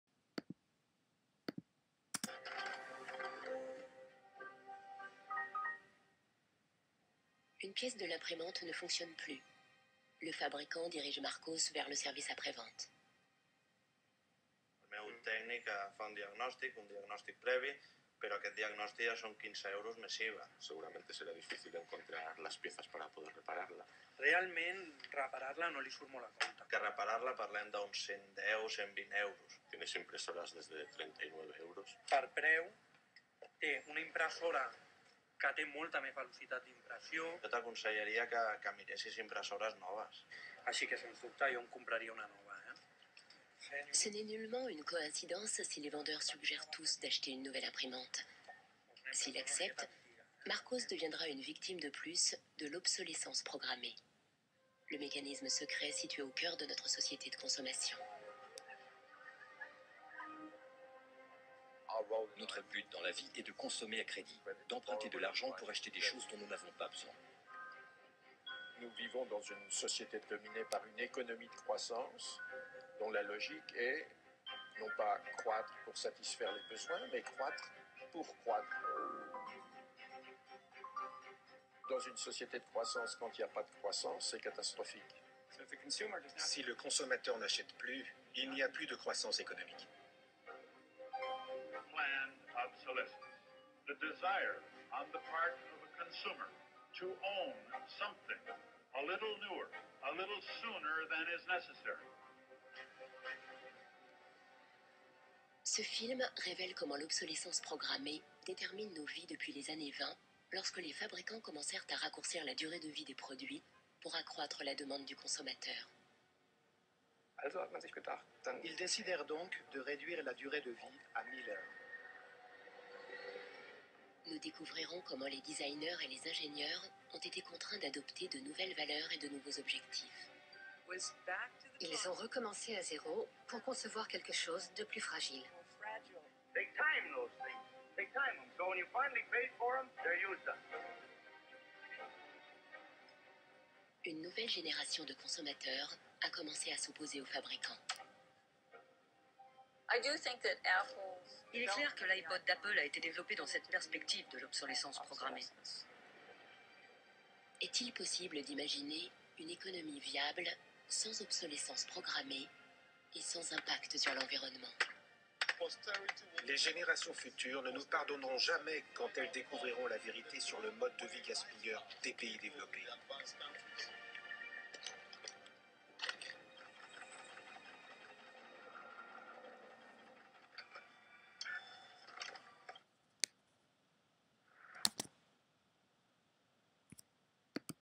L'obsolescence programmée
L'usure prématurée et programmée des objets par les industriels... ou pourquoi je dispose chez moi d'une boîte remplie d'ampoules et de lampes de poches usagées...